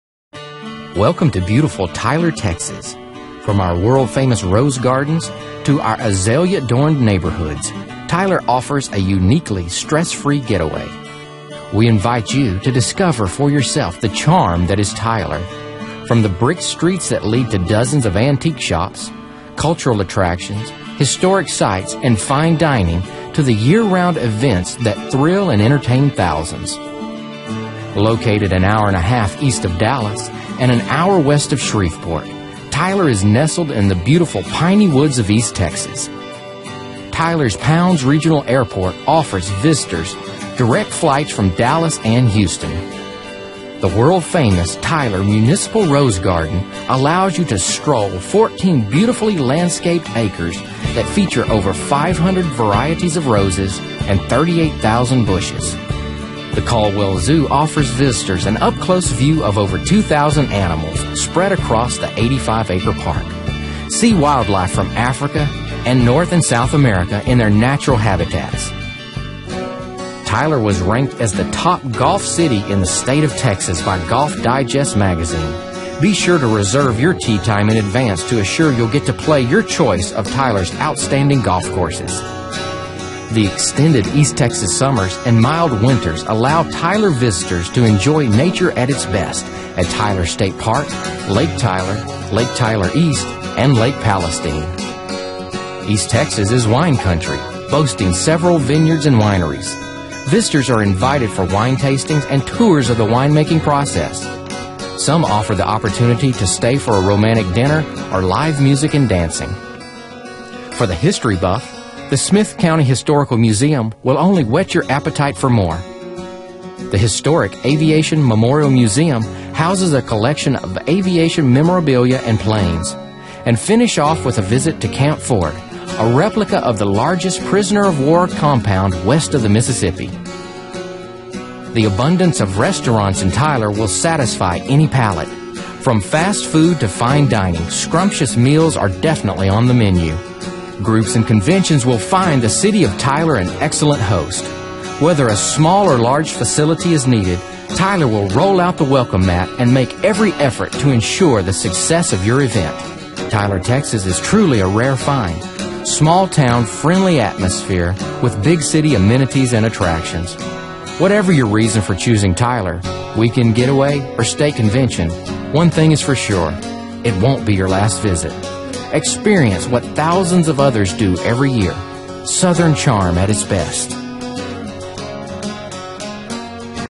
Tyler
Nestled in the piney woods of East Texas, this city is recognized as the Rose Capital of America. Tyler lies within a short distance of several lakes and their surrounding woodlands and meadows. In the spring, the countryside is rich with a colorful and fragrant tapestry of blooming dogwoods, azaleas, roses, and myriads of wildflowers. In the fall, it is ripe with the golds, scarlets, nut browns, and velvet greens of an endless variety of East Texas trees, including oak, dogwood, pecan, pine, sweetgum and sycamore.